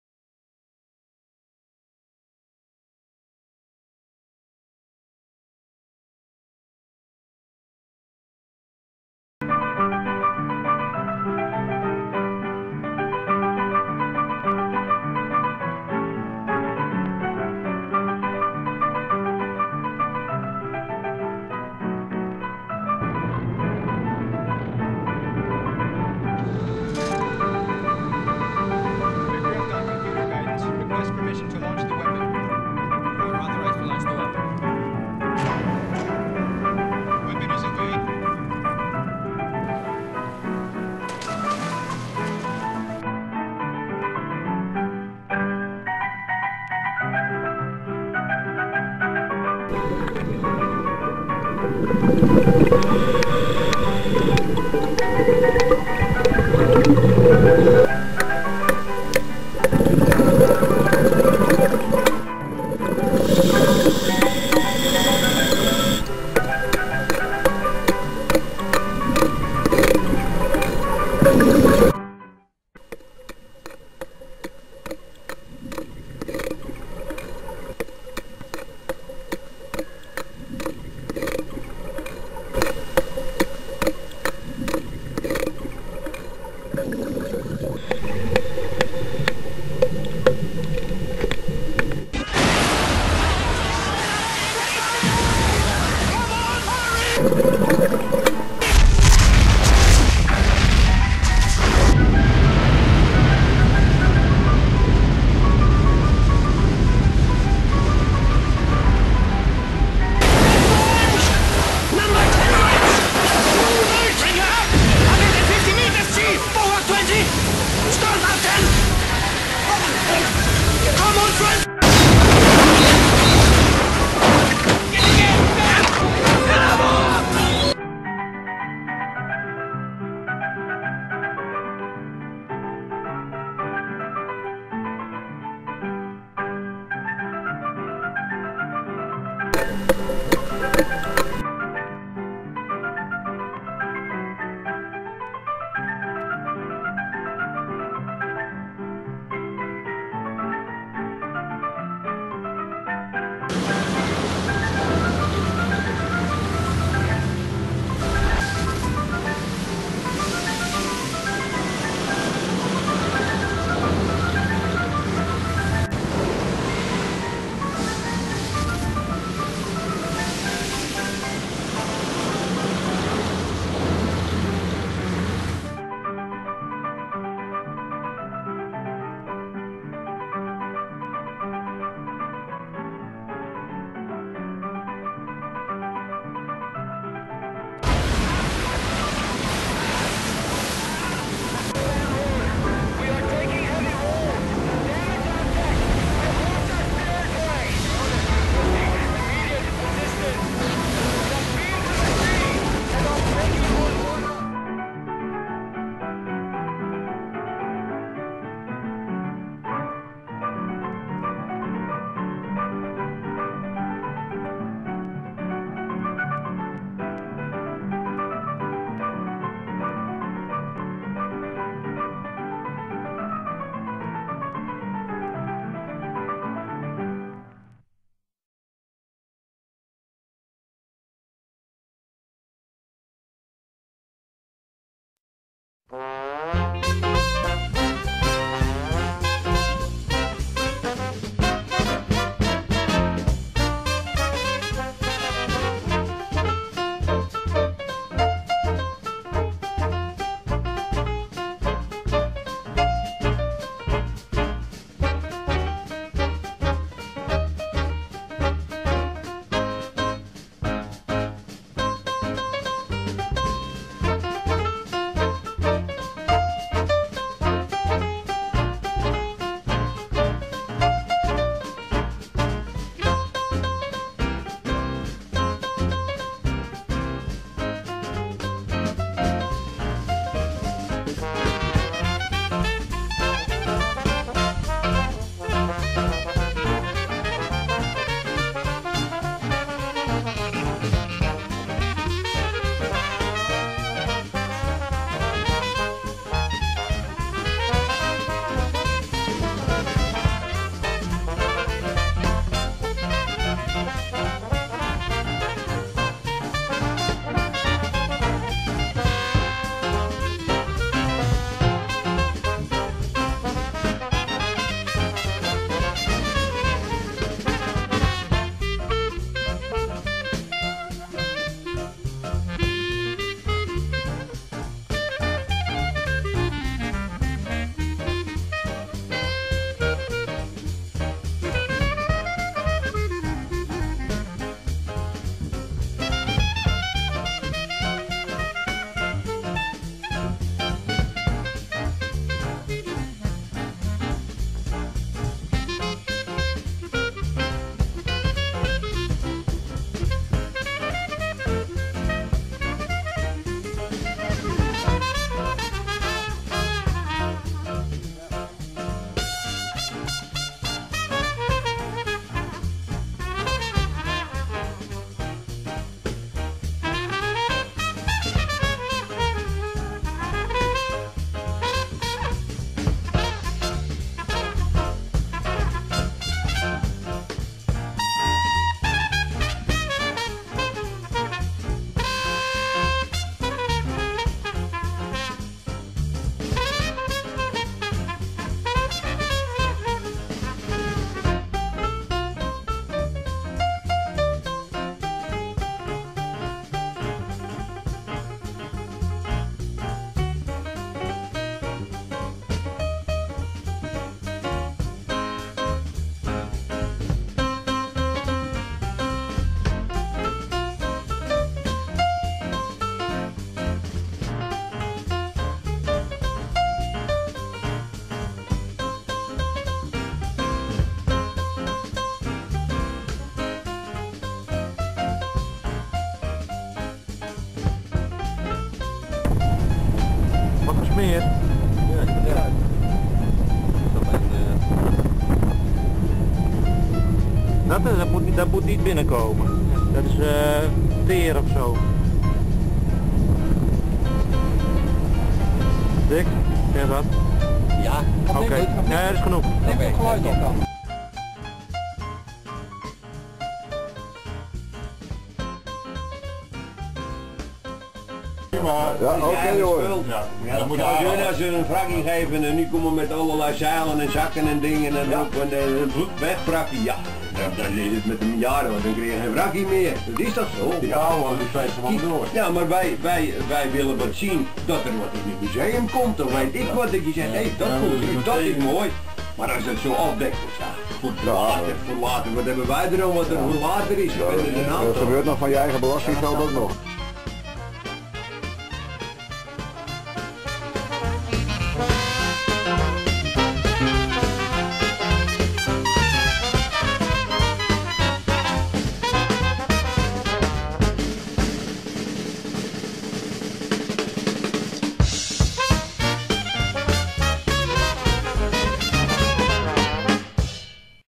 HemtronFilmpje2(2/2)
part 2: Movie made for promoting northsea diving north of the dutch islands. Movie was presented by Hemtron on the yearly video night for the Frisian Scuba Divers who often join the ms.Redbad and the ms.Wilhemina for their nothea scuba dives.